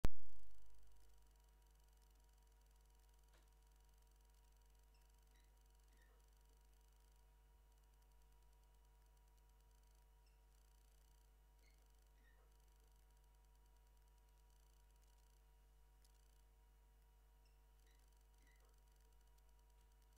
Funny Lithuania : Spain basketball Rudy Fernandez blooper
Funny Lithuania : Spain basketball Rudy Fernandez blooper, Simas Jasaitis assist :DD